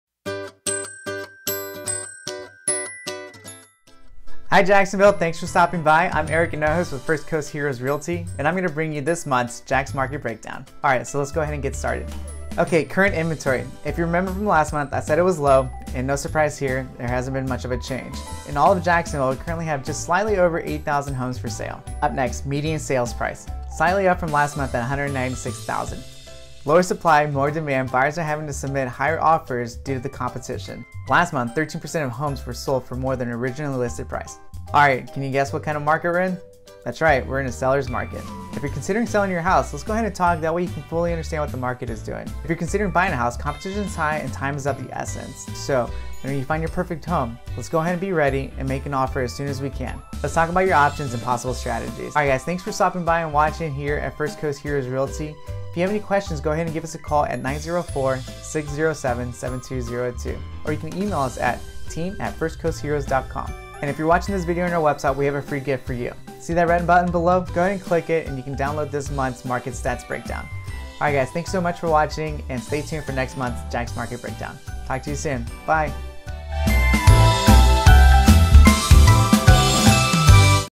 April Market Stats
Check out the latest Jacksonville housing stats for April! Do you know what kind of market we're in?

Broker: Navy to Navy Homes